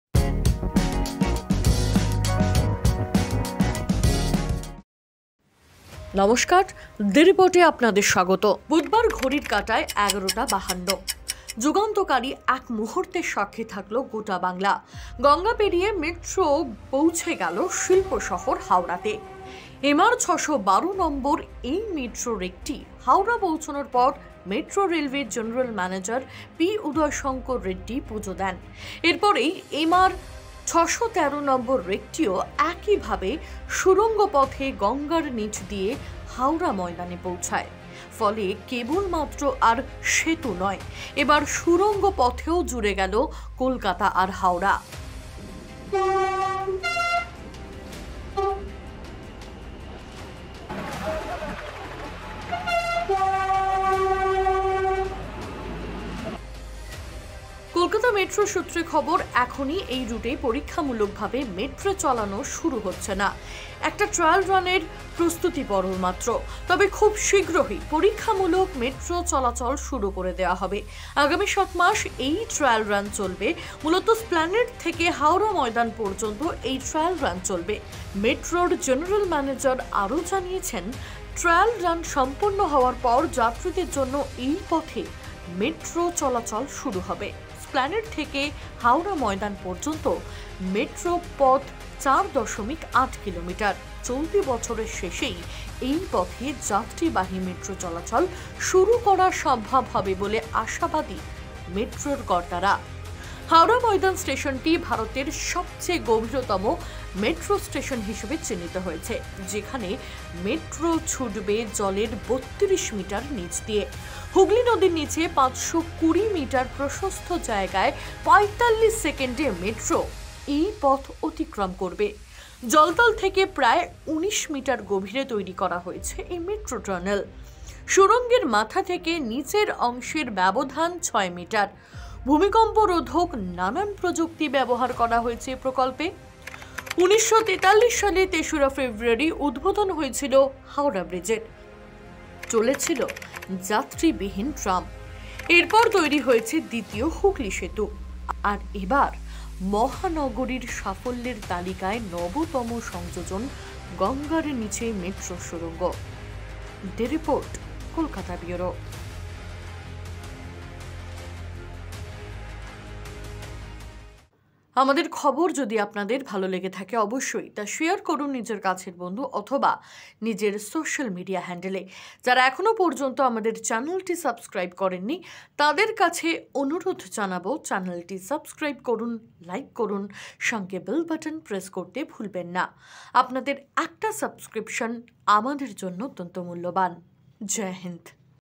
Metro Runs Under River:যুগান্তকারী মুহূর্তের সাক্ষী বাংলা,ইতিহাস সৃষ্টি করে গঙ্গা পেরোলো মেট্রো
যুগান্তকারী এক মুহূর্তের সাক্ষী থাকল বাংলা, ইতিহাস সৃষ্টি করে গঙ্গা পেরোলো মেট্রো